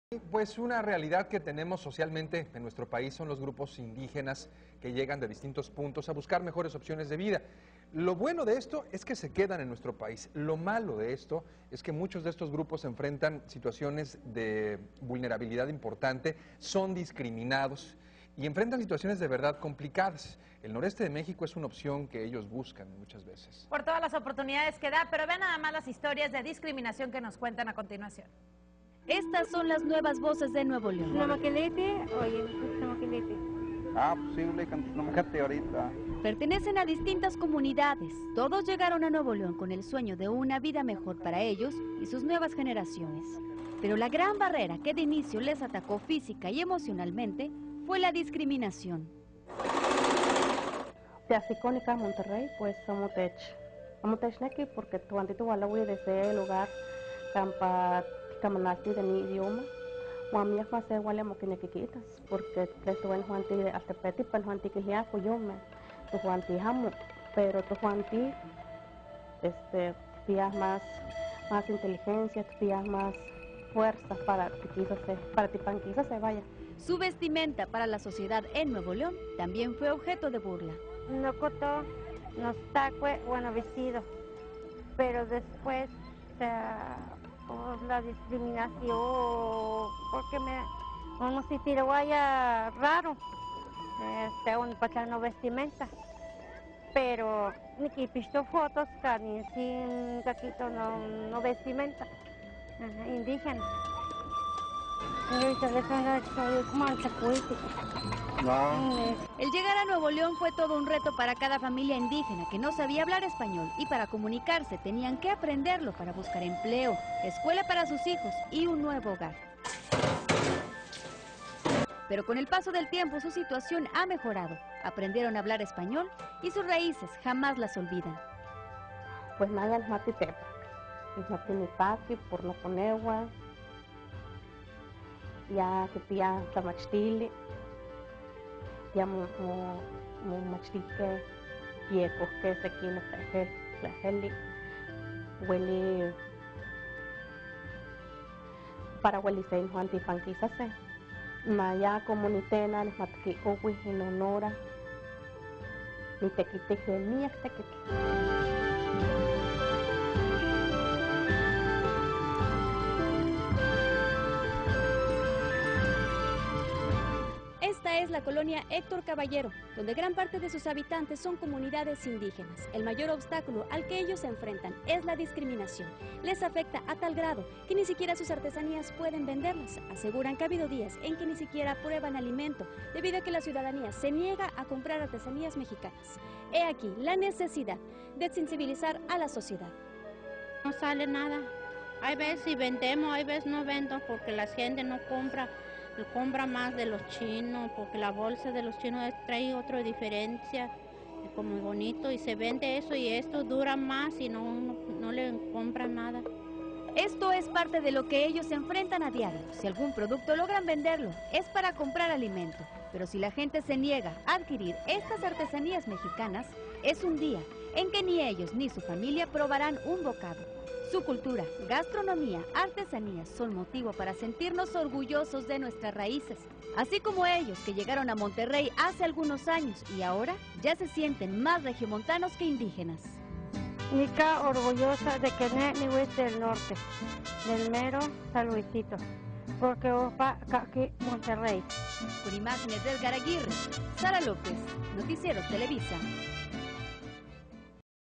Las Noticias - Nuevas voces en Nuevo León
Cultura, gastronomía, artesanías son motivo para sentirnos orgullosos de nuestras raíces.